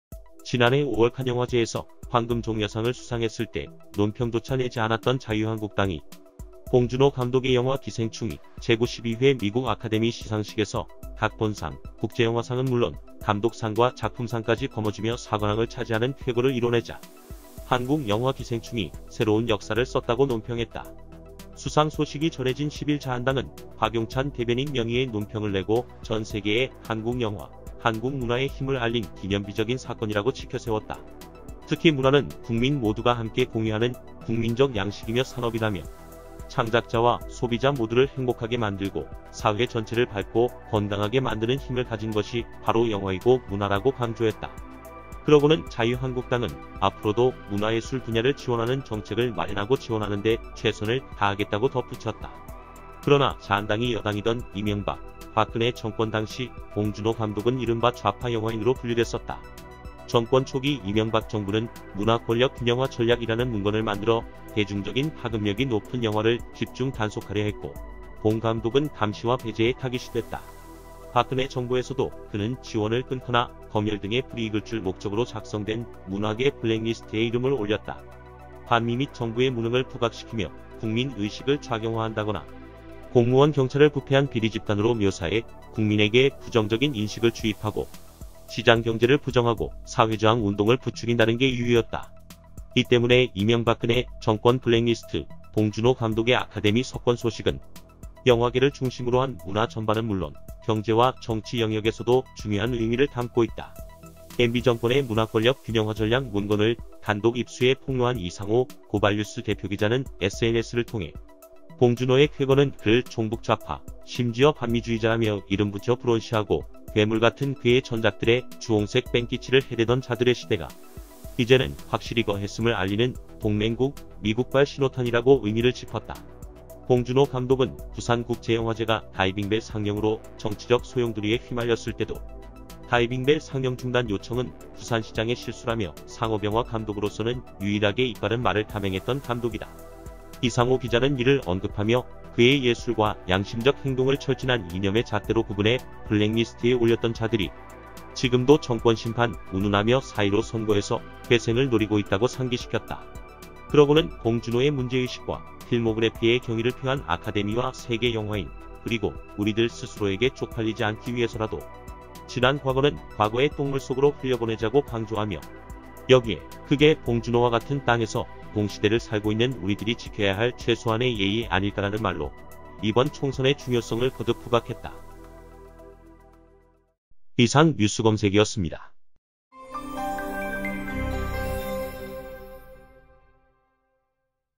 2.11 ‘이명박근혜 블랙리스트’ 봉준호의 아카데미 석권이 갖는 진짜 의미
2.11 ‘이명박근혜 블랙리스트’ 봉준호의 아카데미 석권이 갖는 진짜 의미
[일시구독 후원계좌] KB국민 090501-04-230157 (주)발뉴스

(광고상담 시간: 월~금 오전10시 ~ 오후6시)